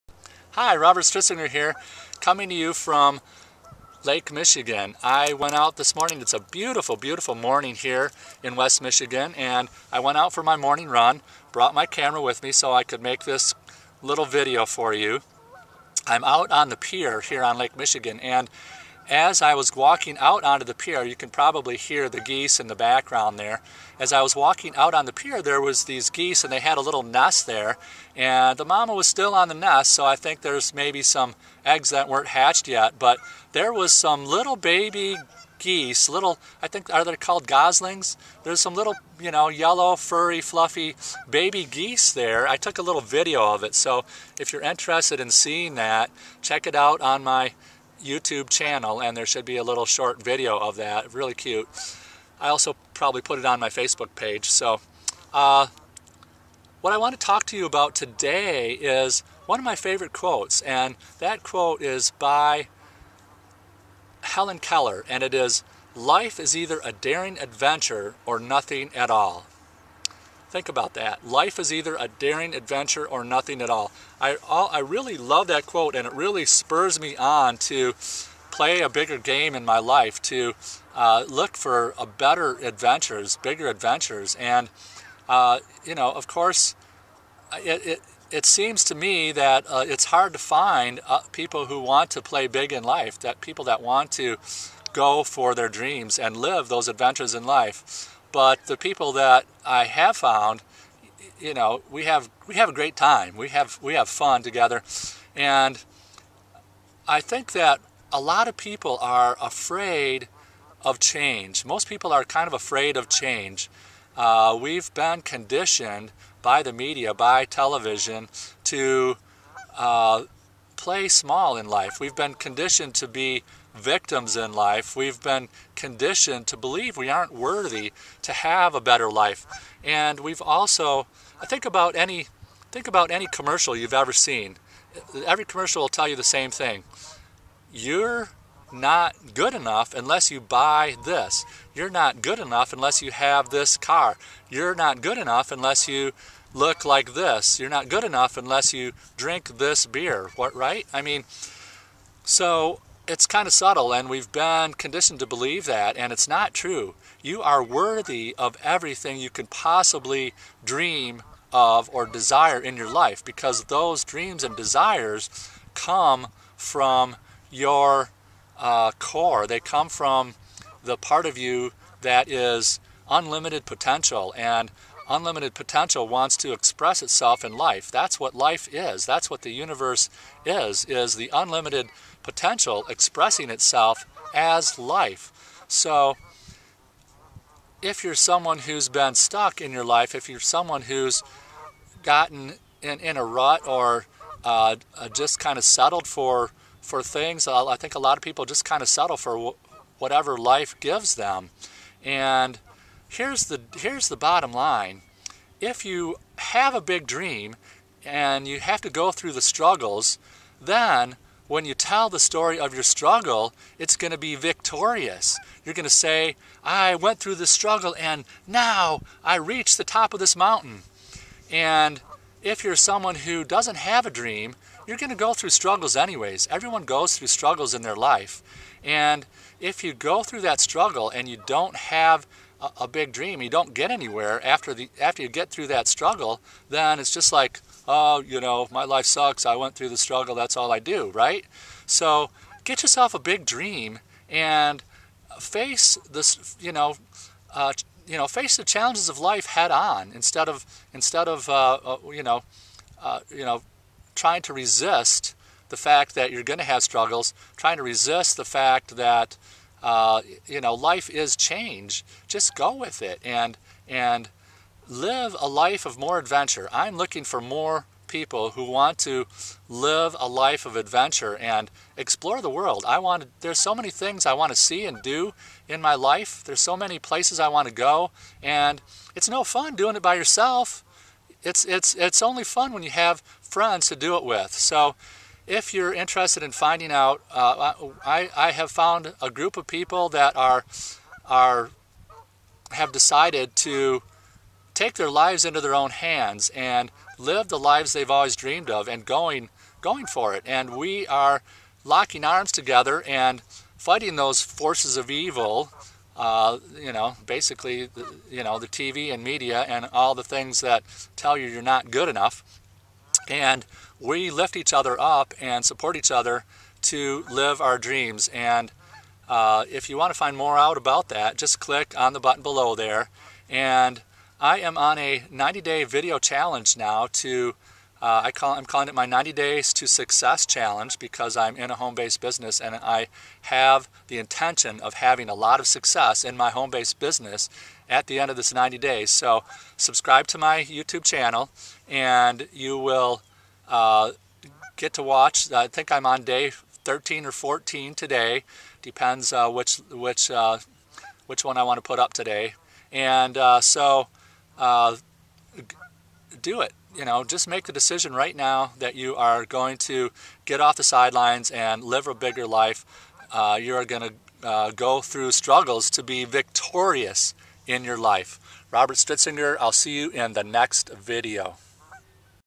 90 Day Success Story Day 13 Make Your Life a Daring Adventure
Step outside your comfort zone.  Get off the sidelines of
your life and get in the game. Helen Keller was determined
to live life to its fullest even though she was deaf, dumb
and blind.

She became a symbol of determination, hope
and strength to many people during her lifetime.

Helen Keller became friends with Mark Twain and
visited every president from the time she was a little girl.

What does it mean to make your Life a daring adventure?

Making your life a daring adventure then, is choosing to
take control of your life and live your life on purpose.

Live Your Life on Purpose

Do you really want to live your life on purpose?
Really? OK THEN!

Right now, today, make the decision to live your
life on purpose, take control of your destiny and
strike out towards your dreams and desires.